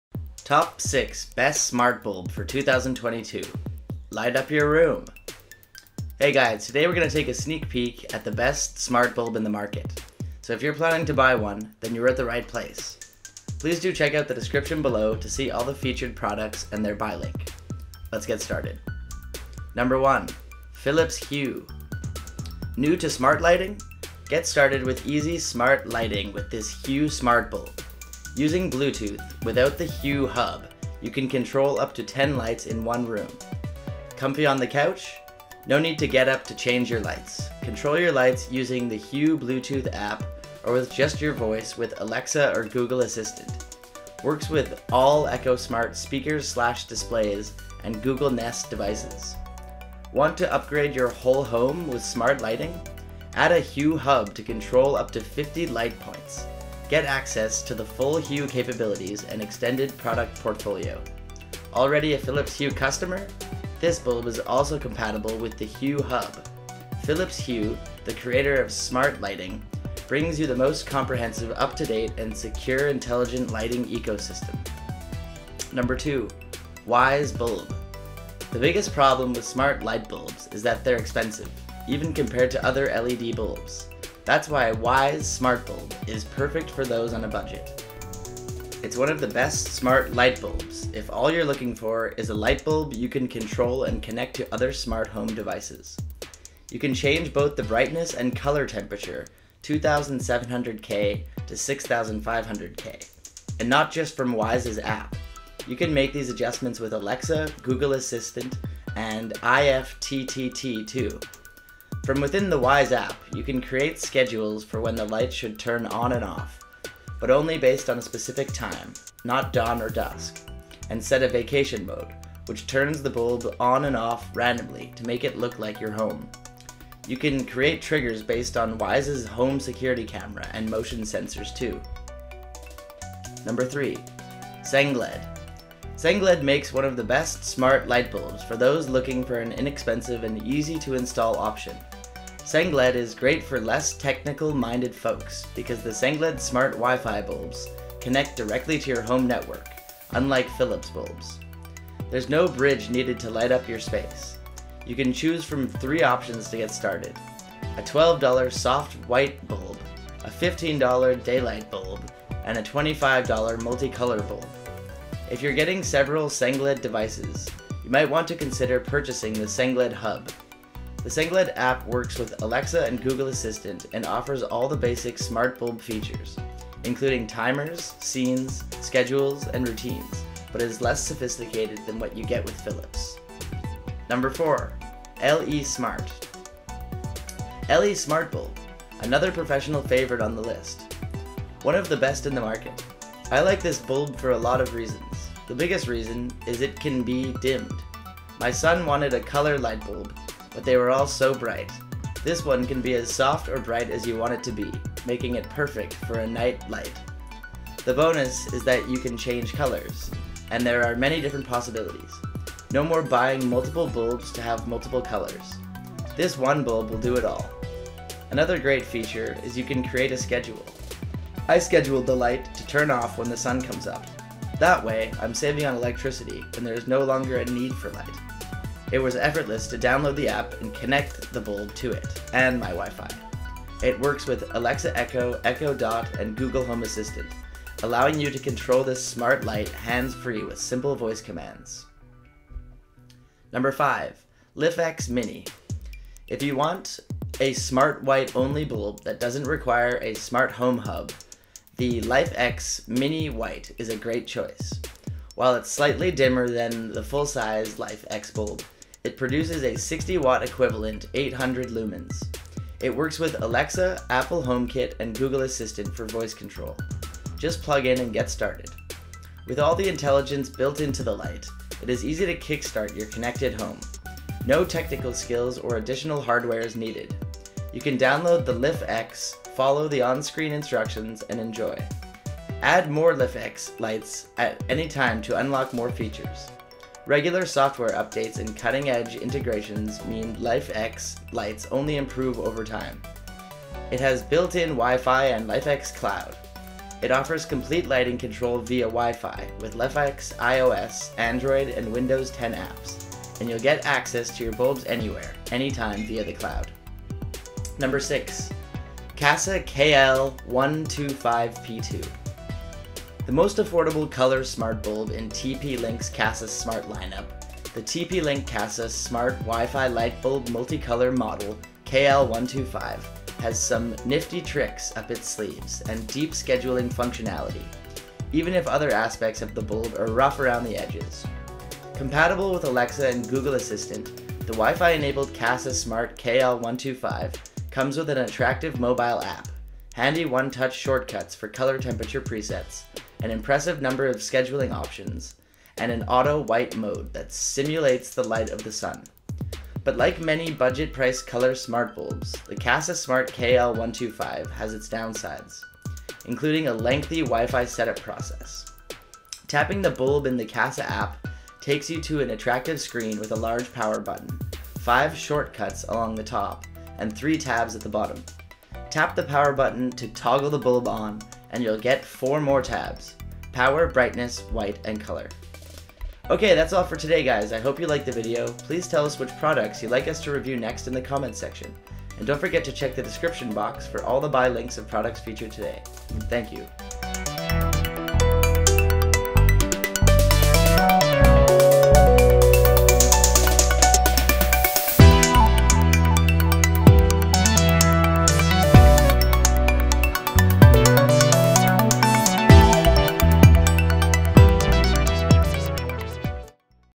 TOP 6: Best Smart Bulb For [2022] - Light Up Your Room!
🠺 Philips Hue
🠺 Wyze Bulb
🠺 LE Smart
🠺 Lifx Mini
🠺 Kasa KL125P2

TIMESTAMPS
00:00 - 00:20 INTRO
00:20 - 01:15 Philips Hue
01:15 - 02:13 Wyze Bulb
02:13 - 03:03 Sengled
03:03 - 04:03 LE Smart
04:03 - 05:07 Lifx Mini
05:07 - 06:16 Kasa KL125P2

Hey guys, today we are going to take a sneak peek at the  Best Smart Bulb  in the market. So if you are planning to buy one, then you are at the right place. Please do check out the description below to see all the featured products and their buy link. let's get started.

1. Philips Hue

New to smart lighting? Get started with easy smart lighting with this Hue smart bulb. Using Bluetooth, without the Hue Hub, you can control up to 10 lights in 1 room. Comfy on the couch? No need to get up to change your lights. Control your lights using the Hue Bluetooth app or with just your voice with Alexa or Google Assistant. Works with all Echo smart speakers/displays and Google Nest devices.

2. Wyze Bulb

The biggest problem with smart light bulbs is that they're expensive, even compared to other LED bulbs. That's why Wyze's smart bulb is perfect for those on a budget. It's one of the best smart light bulbs if all you're looking for is a light bulb you can control and connect to other smart home devices. You can change both the brightness and color temperature 2,700k - 6,500k, and not just from Wyze's app; you can make these adjustments with Alexa, Google Assistant, and IFTTT too.

3. Sengled

Sengled makes one of the best smart light bulbs for those looking for an inexpensive and easy-to-install option. Sengled is great for less technical-minded folks because the Sengled Smart Wi-Fi bulbs connect directly to your home network, unlike Philips' bulbs. There's no bridge needed to light up your space. You can choose from three options to get started. A $12 soft white bulb, a $15 daylight bulb, and a $25 multicolor bulb.

4. LE Smart

Le Smart bulb !!! Another personal favorite on this list. One of the best in the market. I like this bulb for a lot of reasons. The biggest reason is it can be dimmed. My son wanted a color light bulb, but they were all so bright. This one can be as soft or bright as you want it to be, making it perfect for a night light. The bonus is that you can change colors, and there are many different possibilities. No more buying multiple bulbs to have multiple colors.

5. Lifx Mini

If you want a smart white-only bulb that doesn't require a smart home hub, the LIFX Mini White is a great choice. While it's slightly dimmer than the full-size LIFX bulb, it produces a 60-watt-equivalent 800 lumens. It works with Alexa, Apple HomeKit, and Google Assistant for voice control. Just plugin and get started. With all the intelligence built into the light, it is easy to kick start your connected home.

6. Kasa KL125P2

The most affordable Color smart bulb in TP-Link’s Kasa Smart lineup, the TP-Link Kasa Smart WiFi Light Bulb Multicolor model KL125 has some nifty tricks up its sleeves and deep scheduling functionality, even if other aspects of the bulb are rough around the edges.